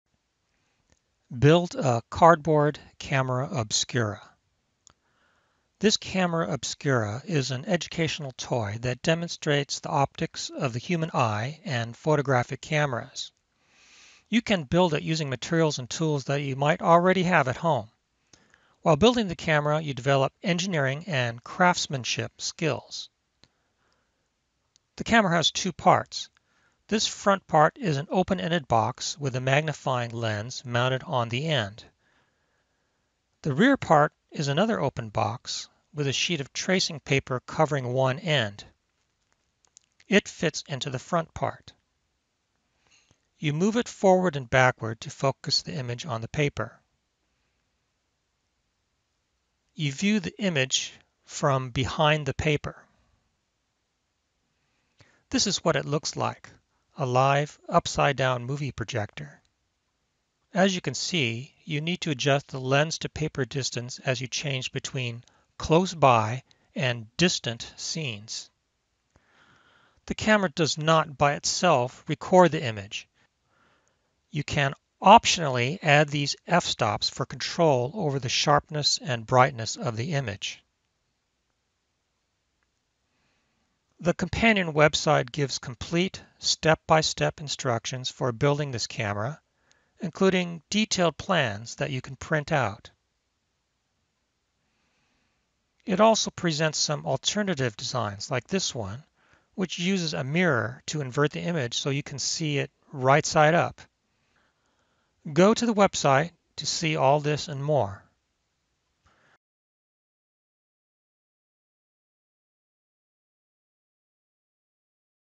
Make a Cardboard Camera Obscura
Build a camera obscura using items you have around the home, and develop engineering and craftsmanship skills.

The camera consists of two open-ended boxes, one with a magnifying lens and the other with tracing paper on the end. One fits inside the other. You move the boxes to focus the image on the paper.

The image looks like a live, upside-down movie projector. The camera does not by itself record the image. In the old days, artists would trace the image using art materials. Nowadays, a digital camera records the image using a light-sensitive electronic sensor.

The companion website gives complete, step-by-step instructions for building this camera, including detailed plans that you can print out.